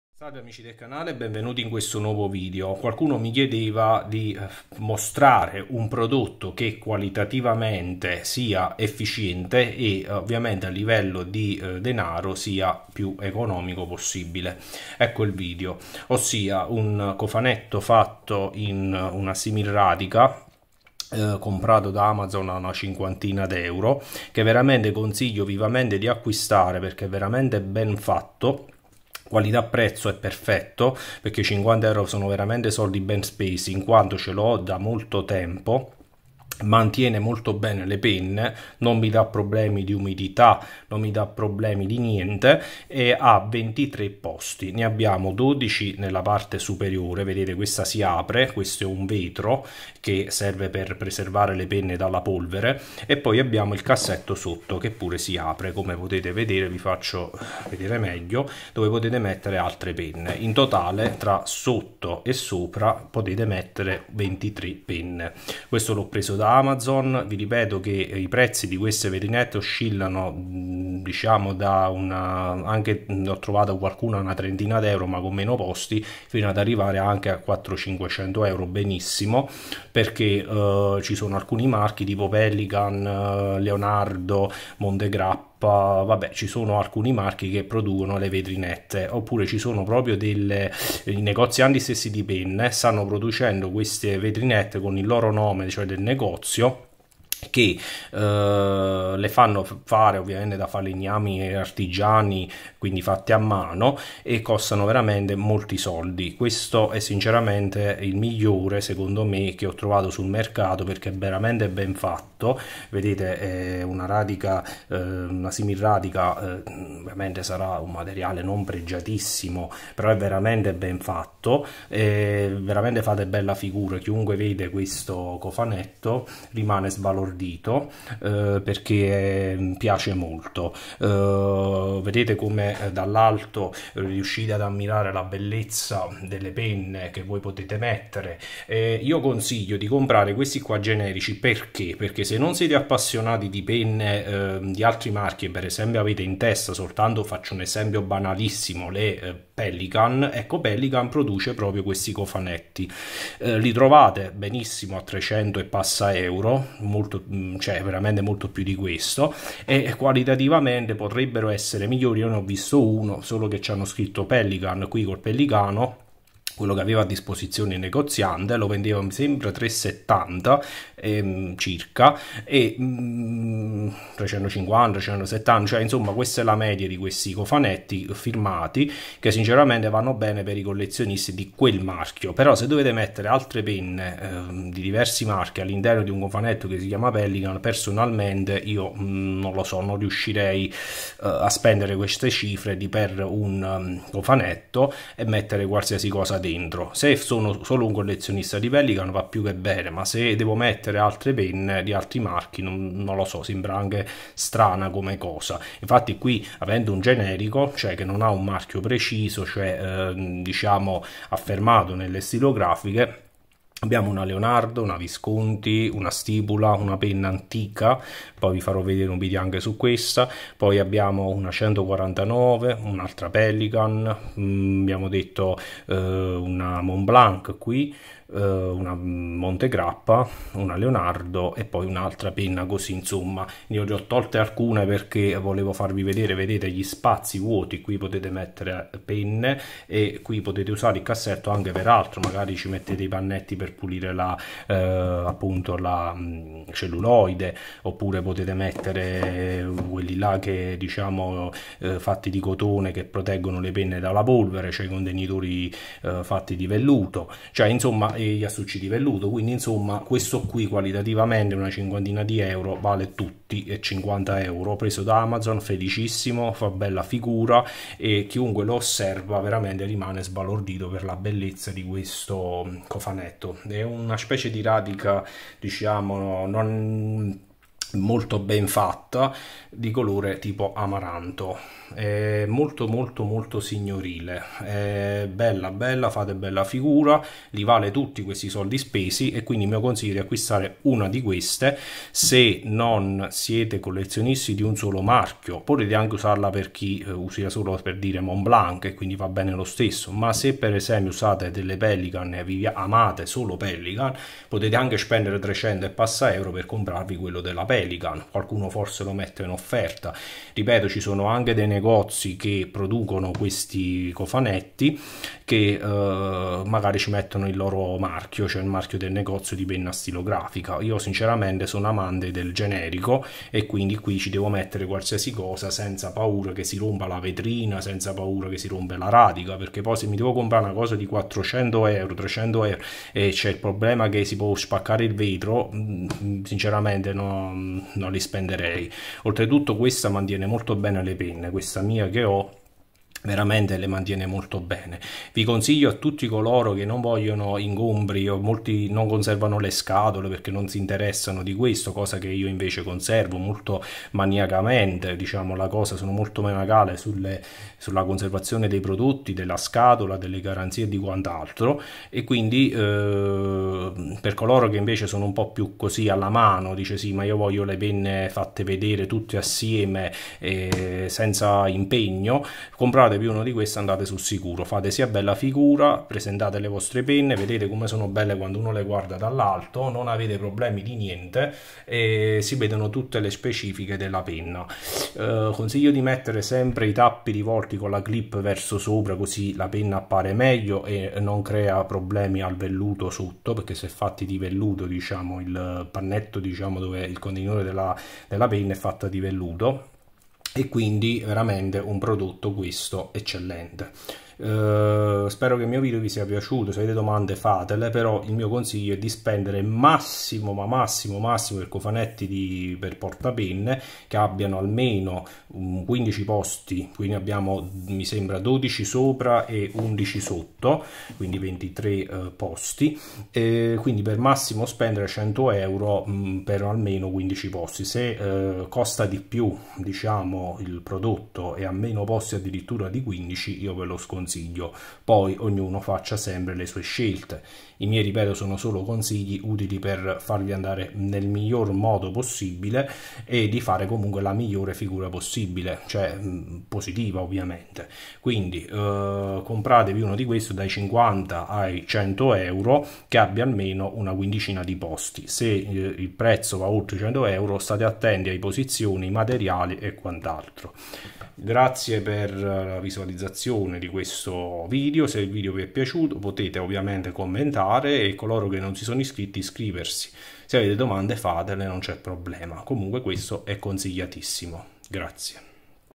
Portapenne con vetrinetta 23 posti.
Ottimo portapenne con vetrinetta 23 posti sia per qualità che per prezzo. Elegante, materiali discreti e non necessita di particolari manutenzioni.